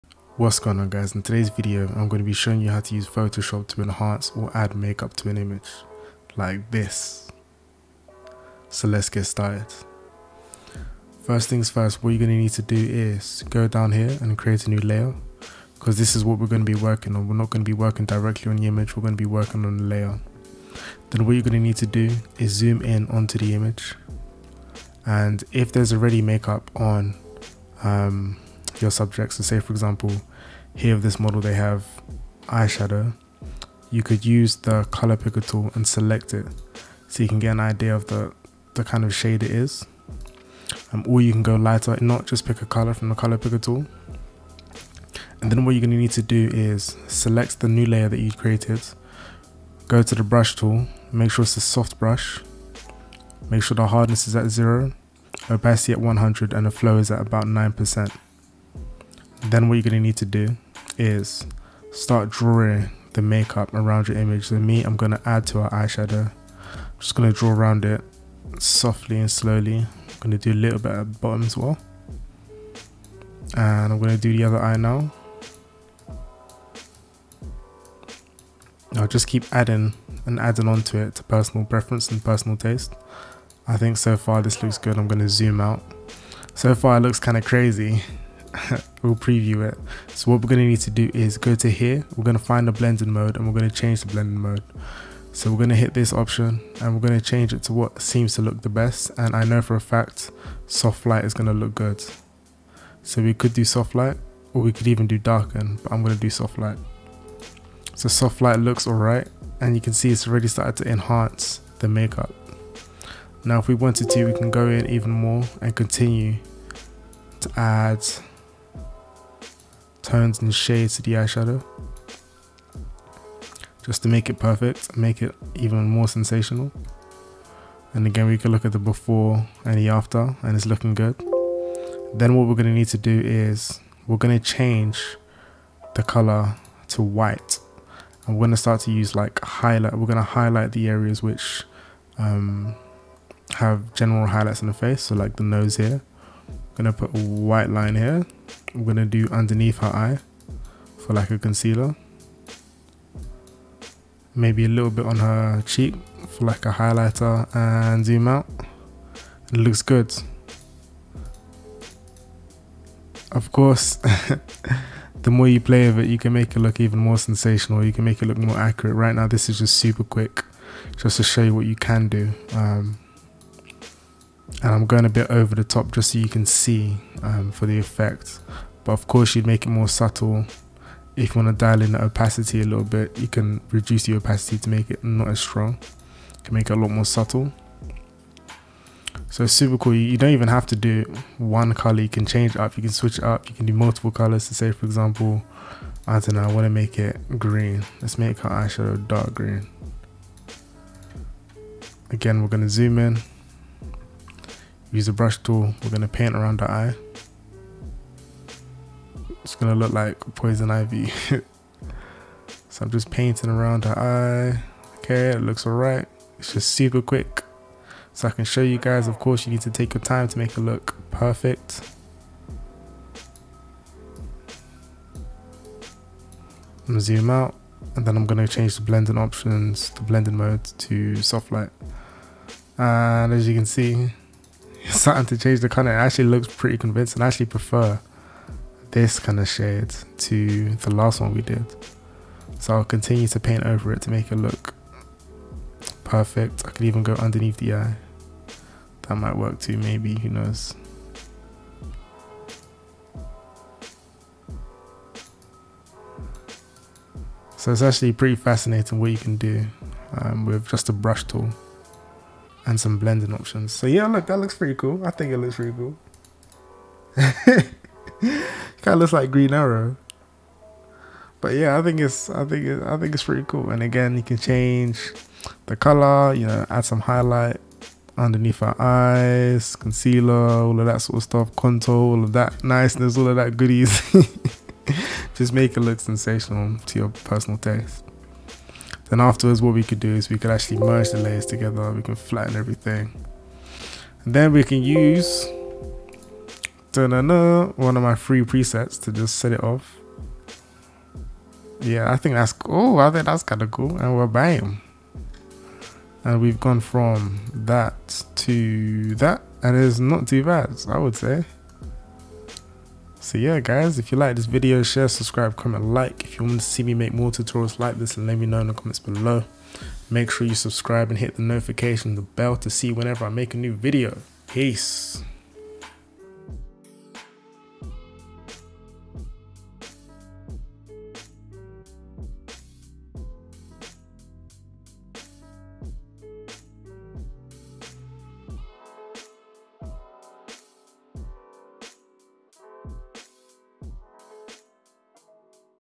Retouch & Enhance Make Up With Photoshop
In this video I'll be showing you a quick technique you can use to retouch and enhance make up using photoshop cc!